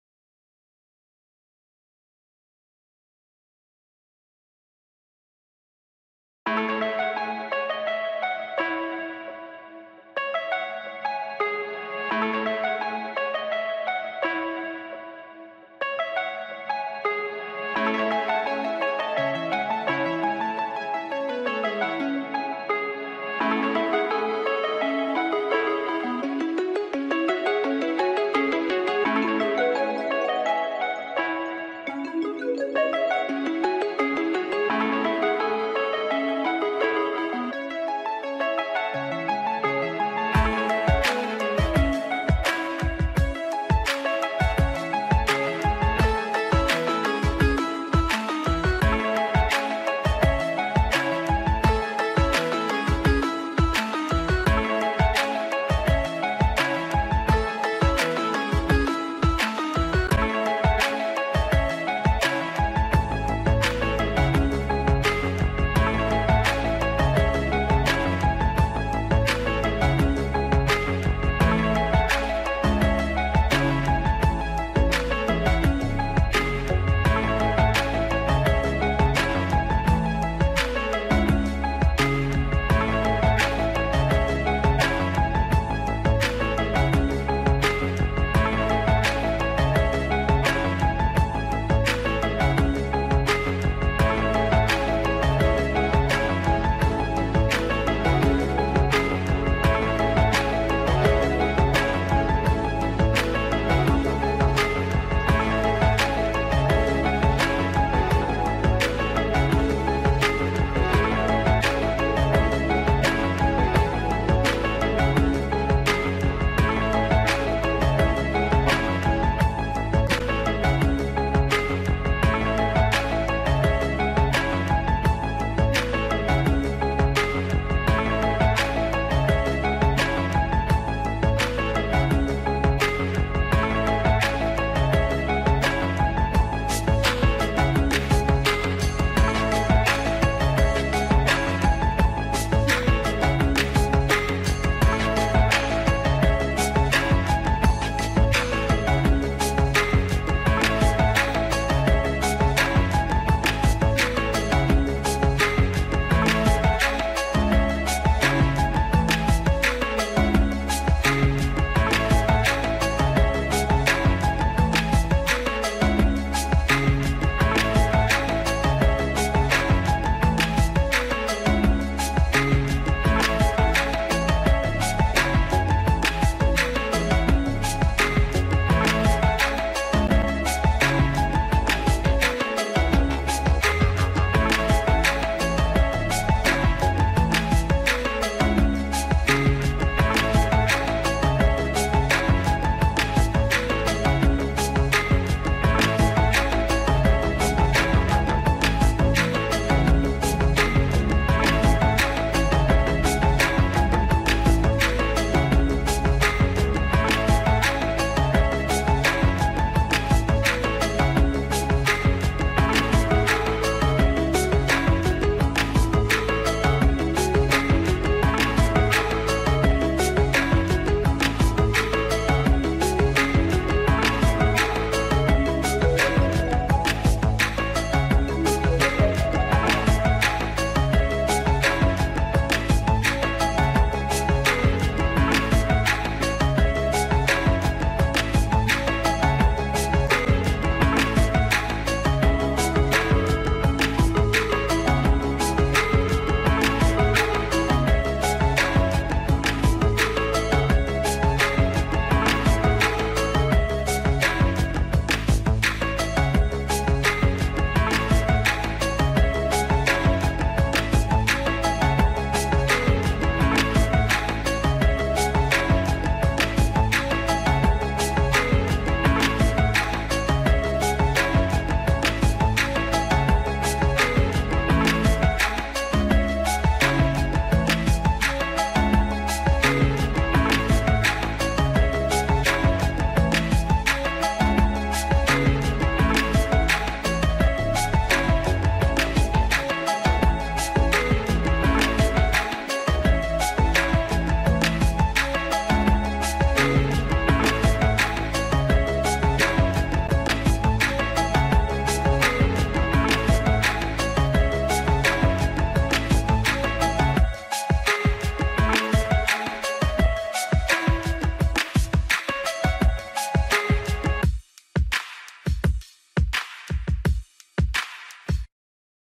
MAYA House Modeling - WALL 02
3D House Modeling - WALL 02
Reference found from Shutterstock.com
Music created from GROOVE PAD